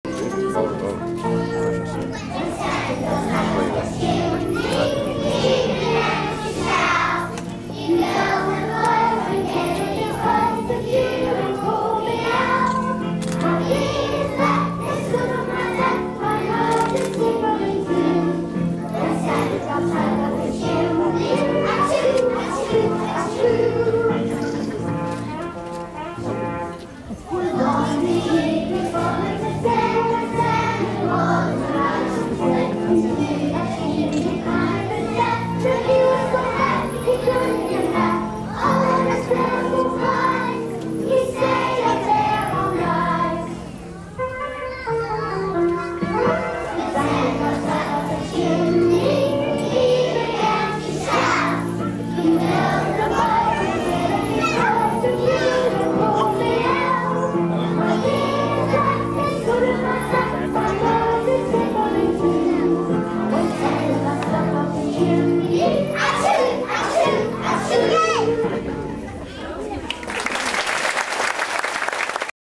Justins Xmas Pagent Song Dec 2008
Justin is wearing the Green hat. This is probably the only time he has ever really participated in one of these events.